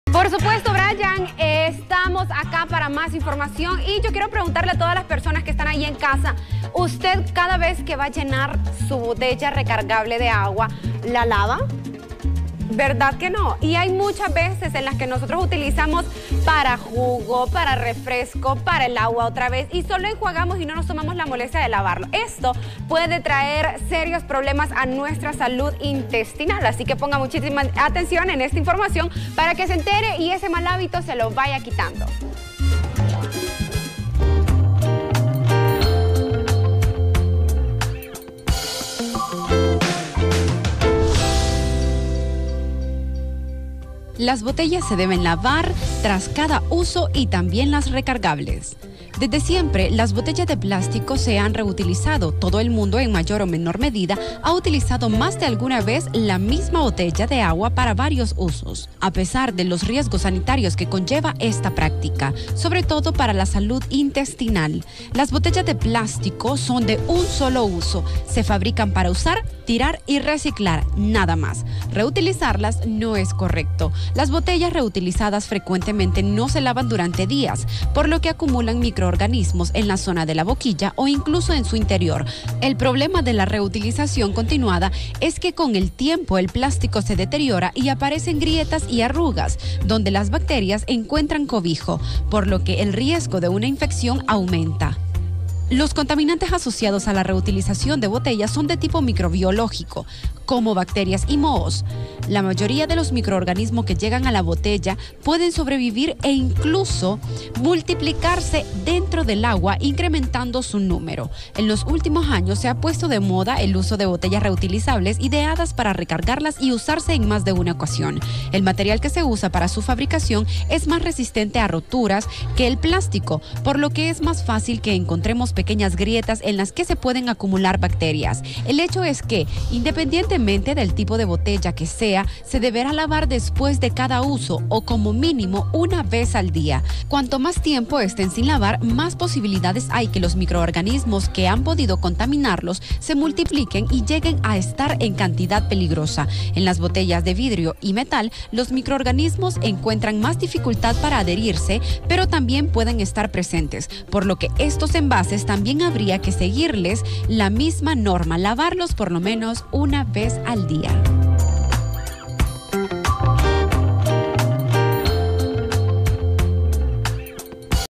Salud al día: Los peligros de no lavar las botellas para el agua
Esta situación puede traer problemas para la salud intestinal ▂▂▂▂▂▂▂▂▂▂▂▂▂▂▂▂▂▂▂▂▂